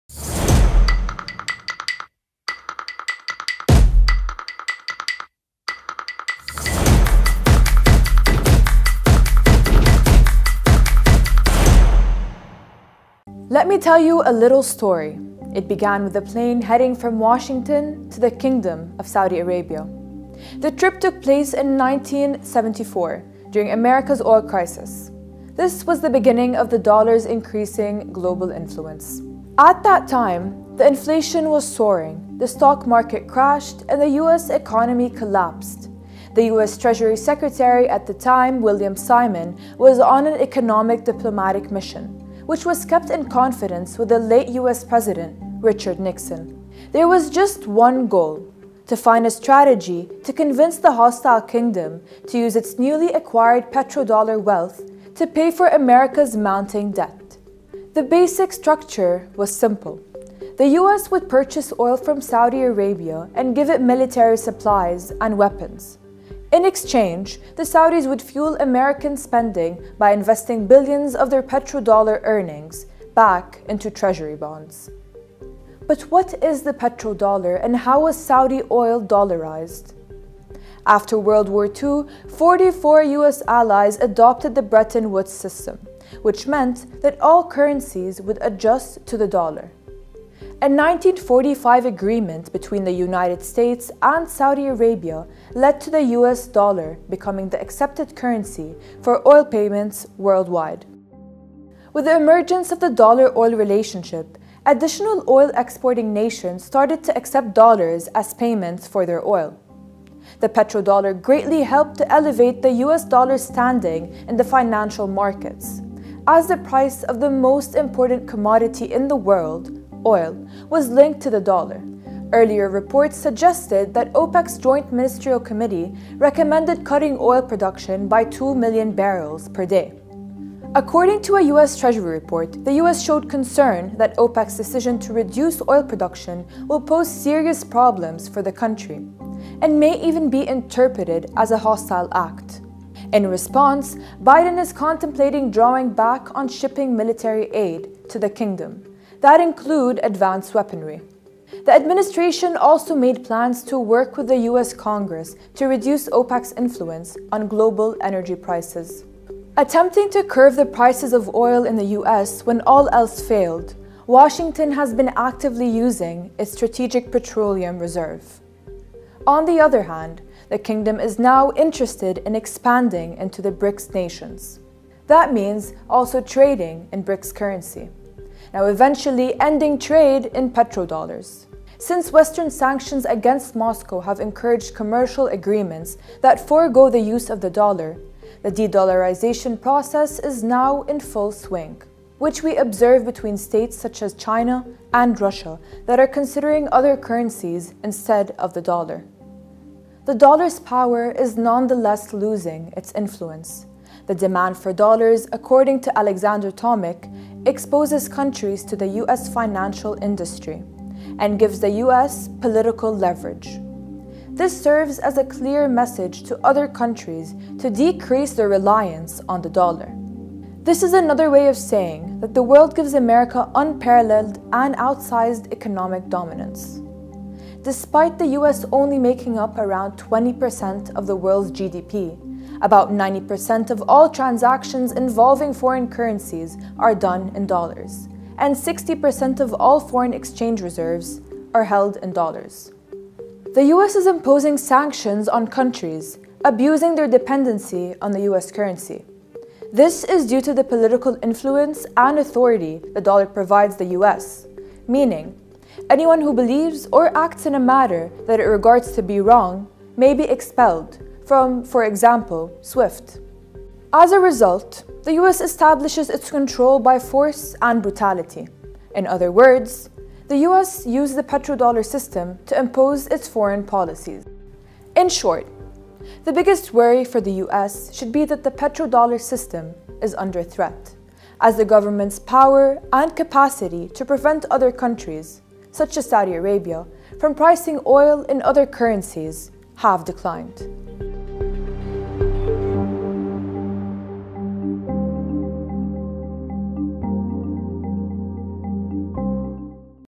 In Five: De-dollarizing oil
The rift between Saudi Arabia and the United States deepens, while the dollar is gradually losing dominance in the US' most prized commodity, oil. Watch to know more about the petrodollar and how the Saudis are prone to ditching the dollar, In Five.